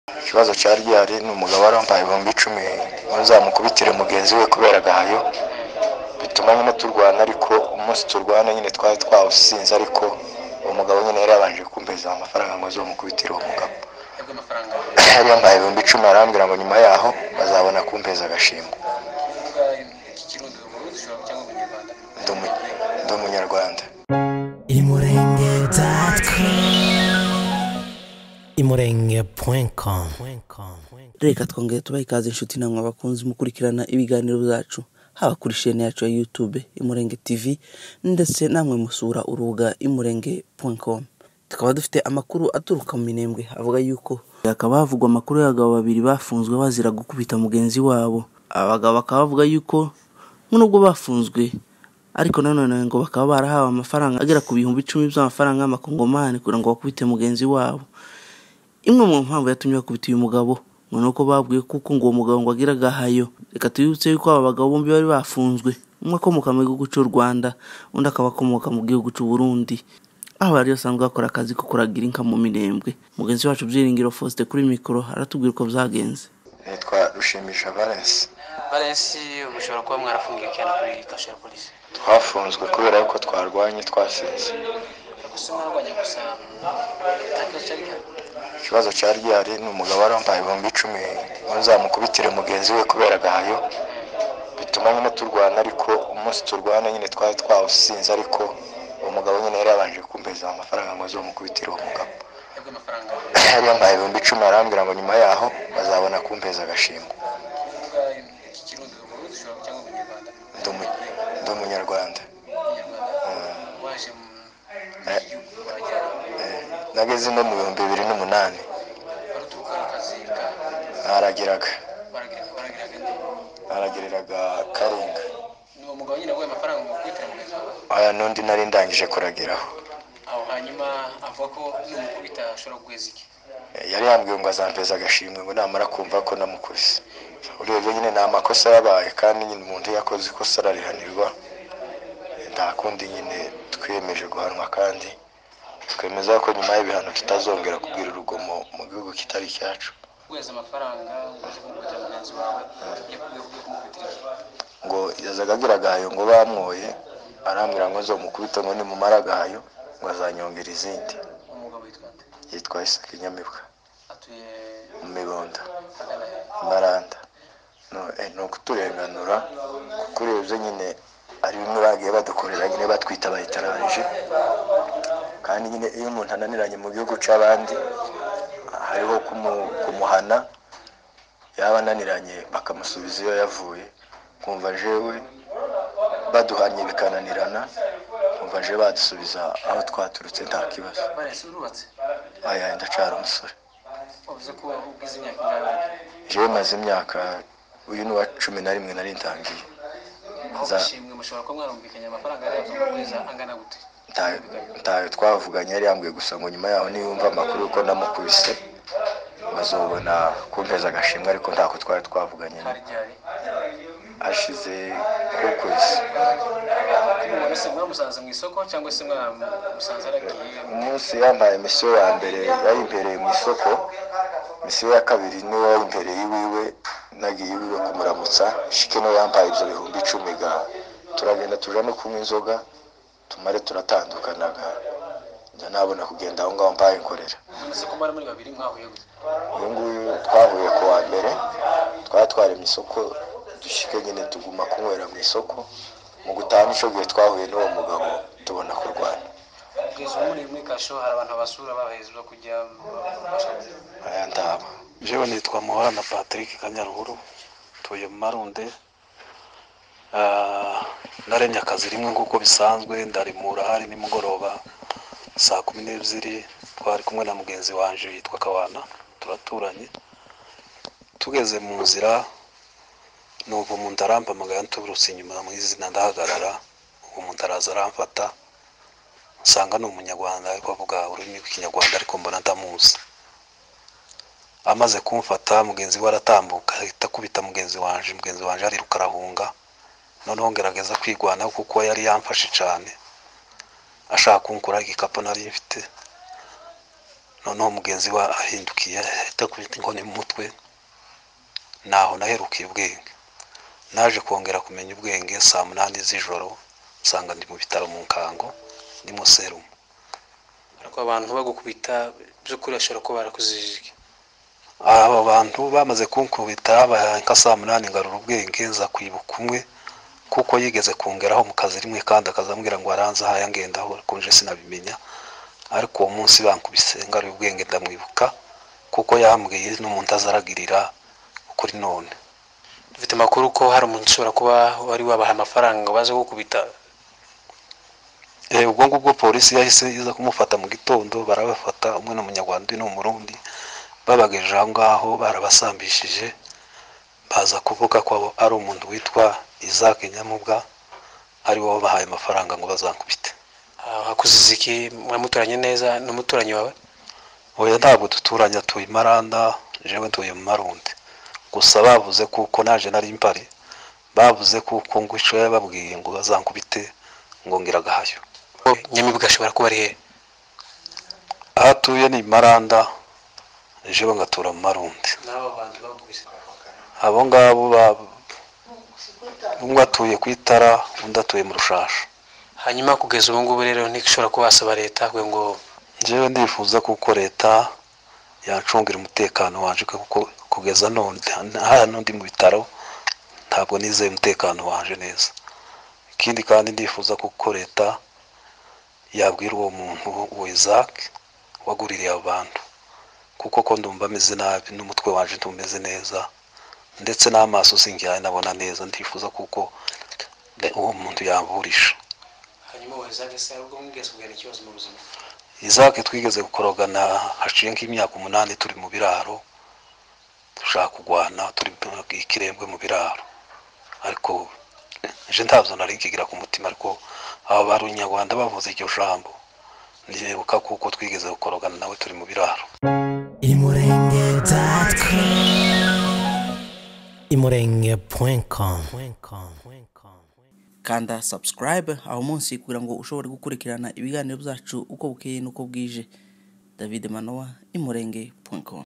Abagabo 2 mu Minembwe bahawe amafaranga ngo bakubite undi mugabo kuko ngo agira agahayo
MINEMBWE, SUD-KIVU – Ku minsi 22/04/2019 mu Minembwe haravugwa amakuru y’abagabo babiri, umwe ukomoka mugihugu c’Urwanda na mugenziwe ukomoka mugihugu c’Uburundi bahawe amafaranga agera ku bihumbi cumi by’amakongomani (10,000 Francs) ngo bakubite undi mugabo w’Umunyamulenge kuko ngo ari umunyagahayo.